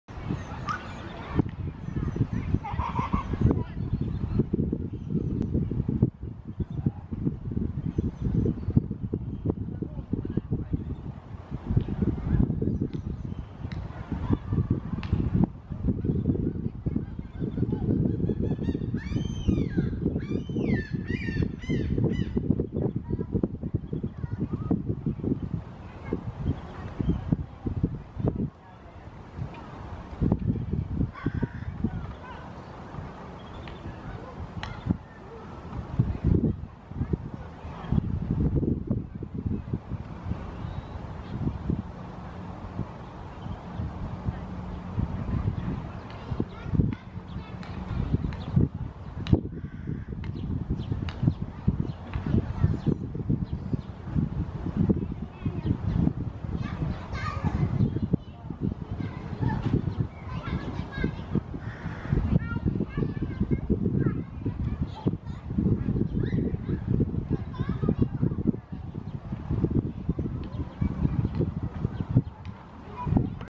Oystercatcher teaching its fledgling to hunt earthworms
Eurasian Oystercatcher (Haematopus ostralegus) teaching its fledgling to hunt earthworms.

Meriharakka (Haematopus ostralegus) opettaa maastopoikaselleen, miten saalistetaan lieroja.